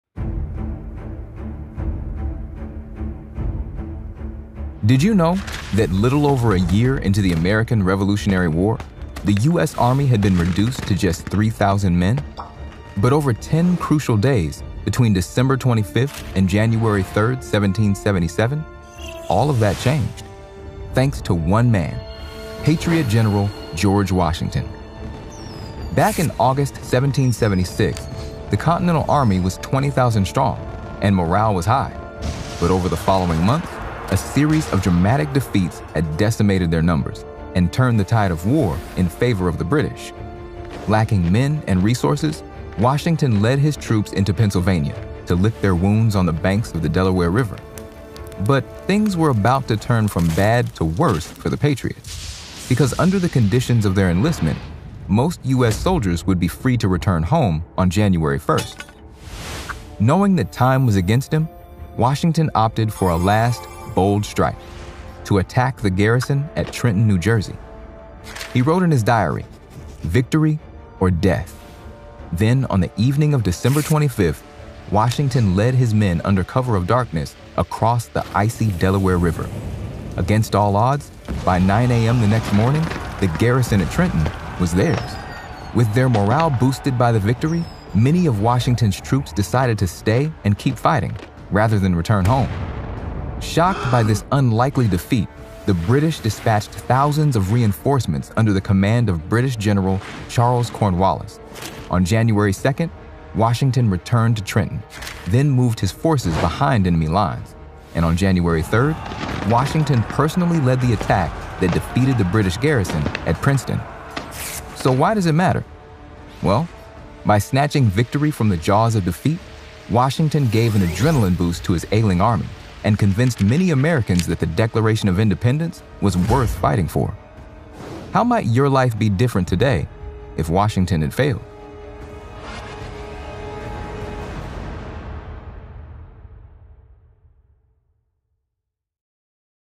The Ten Crucial Days That Changed the American Revolution
December 25, 1776: Victory or Death. General George Washington opted for a last bold strike against the British by crossing the Delaware River and attacking the garrison at Trenton, NJ. This result convinced many Americans that the Declaration of Independence was worth defending.

This video is part of the series How We Became America: The Untold History.  The series is designed to fill in the gaps and bring new stories to life.